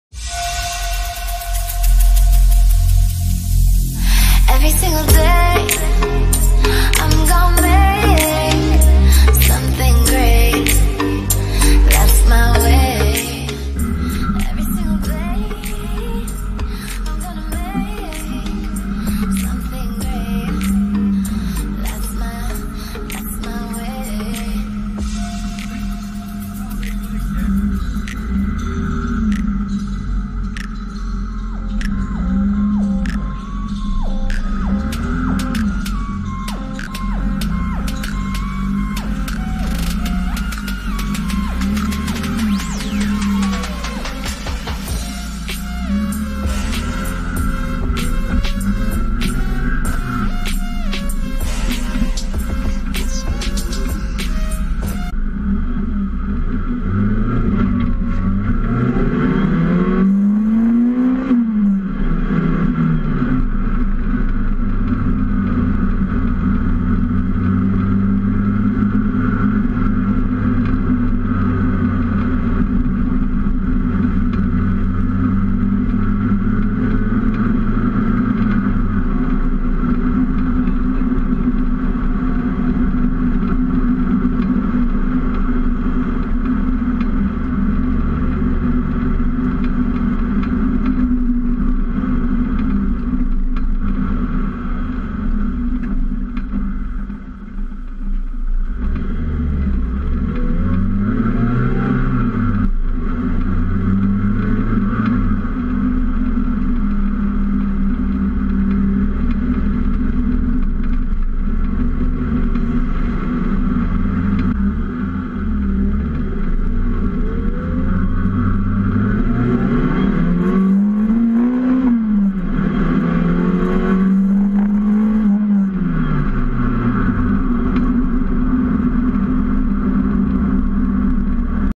Toyota Mr2 mk3 2zz conversion first run out
Sorry bit of a boring video for anyone except me. Built at home in may garage, it's a fully caged sprint car. This is first runs. Stopping checking everything etc etc. No funny business.. It'll be on track soon!!